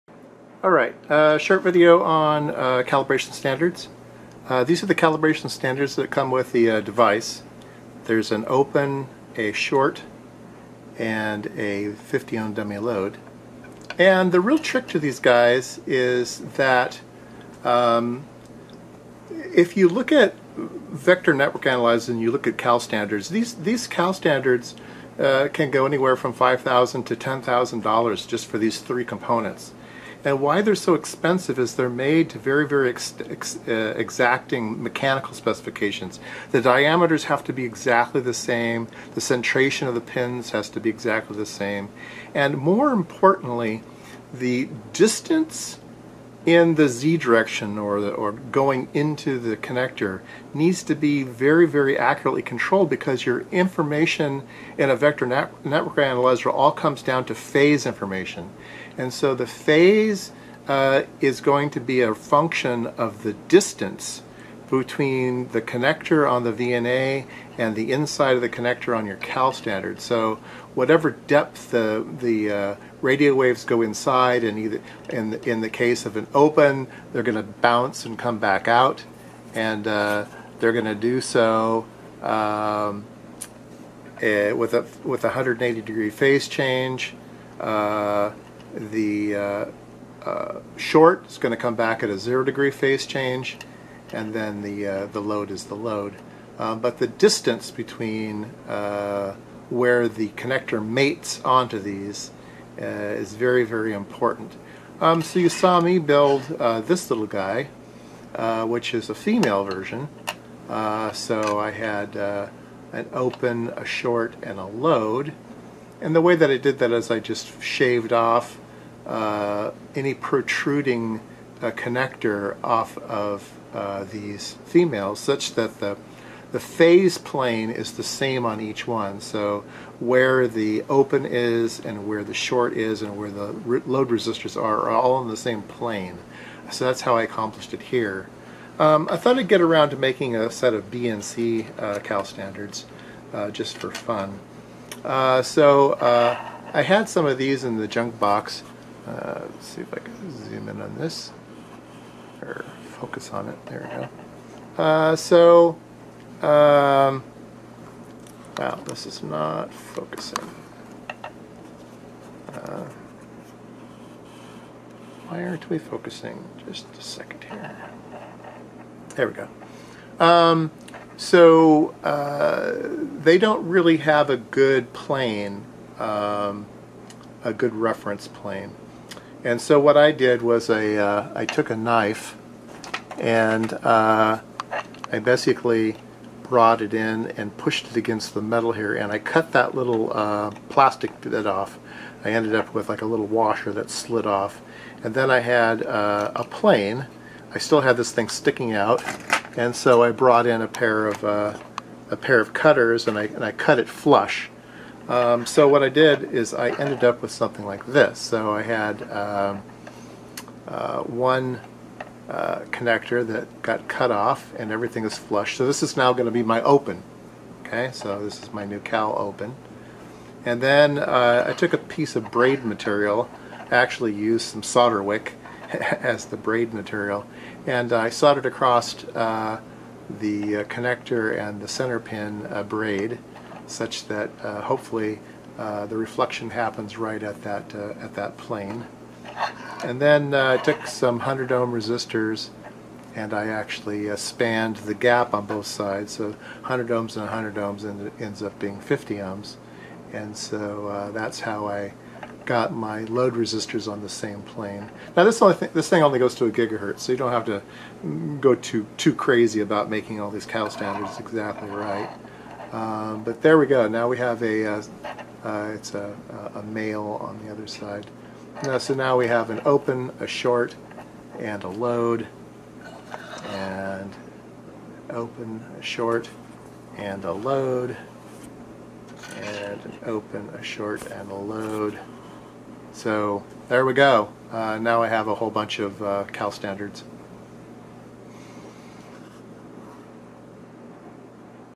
#374 NANOVNA making BNC VNA calibration set
Episode 374
just a note. the short should cover the entire end, using a braid that only covers  half of it is sloppy but  at 900mhz it works just fine.